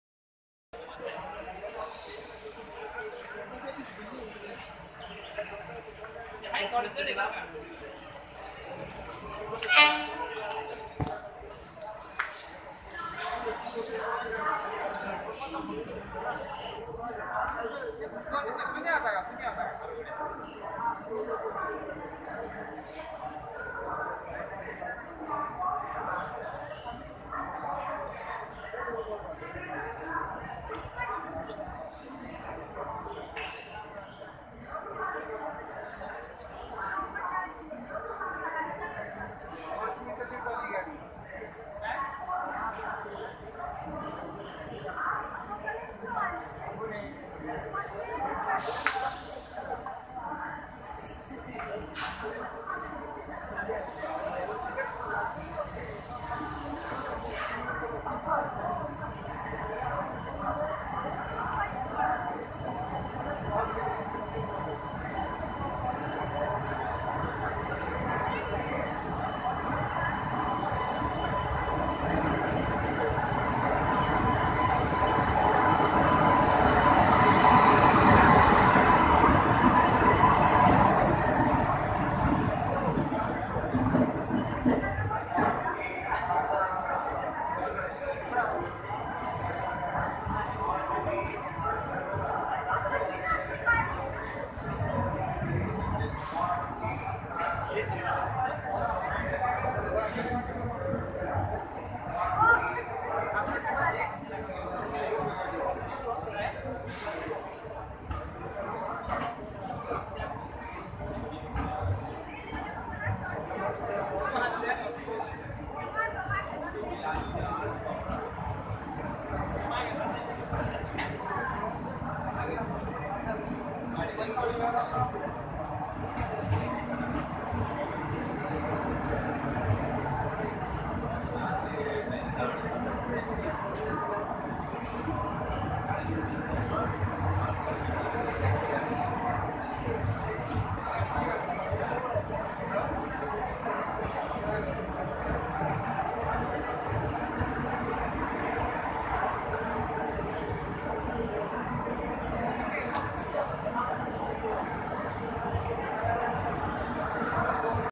Grand entry of 02163 Chennai special superfast Express at Pune Junction
02163 Chennai special superfast express arrival at Pune Junction with WAP7